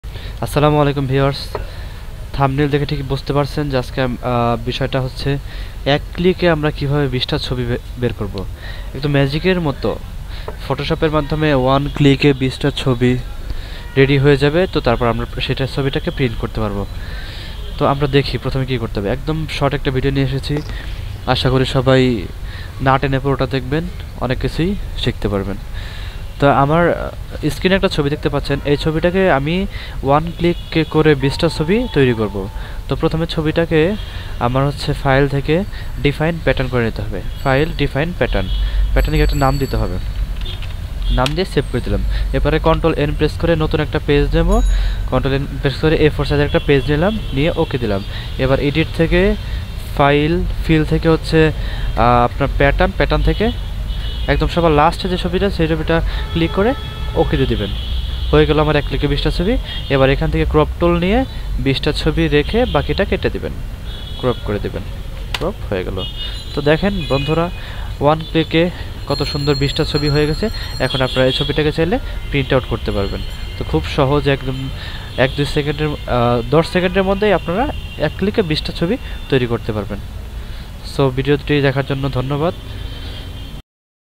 1 Click 20 Passport Size Photo/ মাত্র একটি ক্লিকেই 20 টি পাসপোর্ট সাইজ ছবি
খুব সহজেই ফটোশপ দিয়ে এক ক্লিকে 20 টা ছবি করা যায়।
যদি ভিডিও দেখার পর কোন সমস্যা থাকে অবশ্যই কমেন্ট সেকশনে জানাবেন।